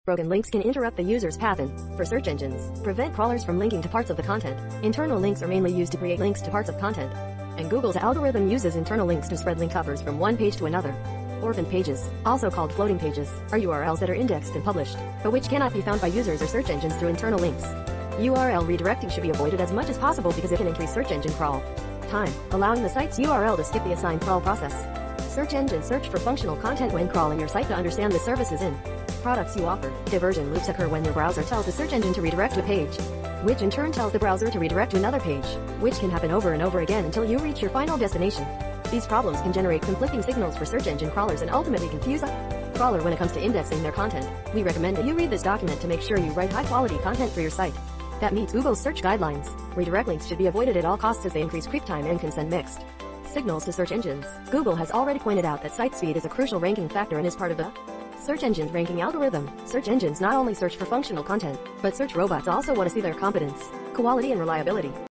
Search Engine Watch - Broken links can interrupt the user's
Broken links can interrupt the user's path and, for search engines, prevent crawlers from linking to parts of the content. Internal links are mainly used to create links to parts of content, and Google's algorithm uses internal links to spread link offers from one page to another. Search SearchEngineWatch Dr Andrew Lipton